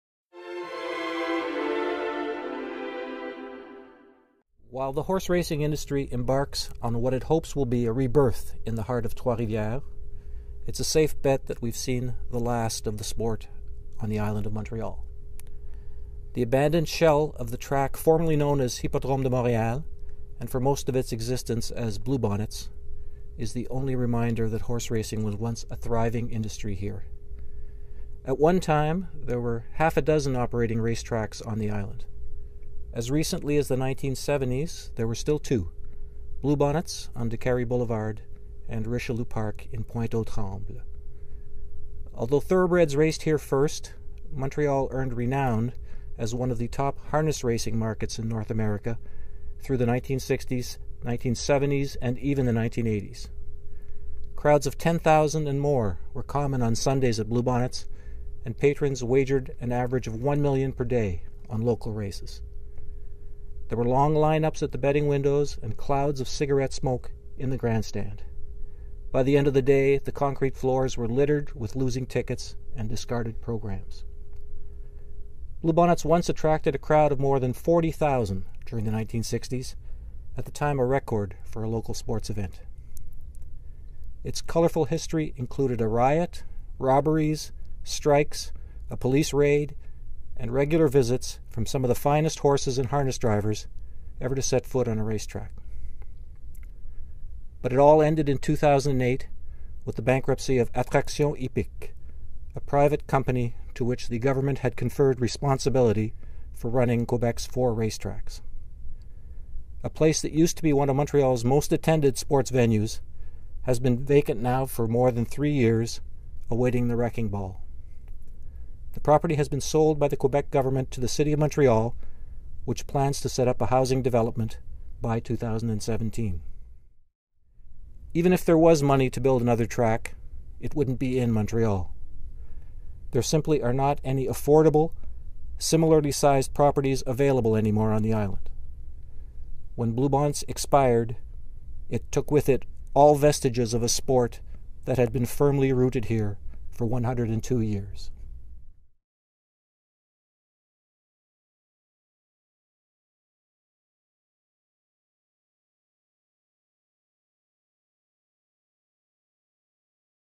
The decline of horseracing in Montreal
An audio slideshow recounting the glory days of what is now merely a memory. Narrated by journalist Paul Delean / THE GAZETTE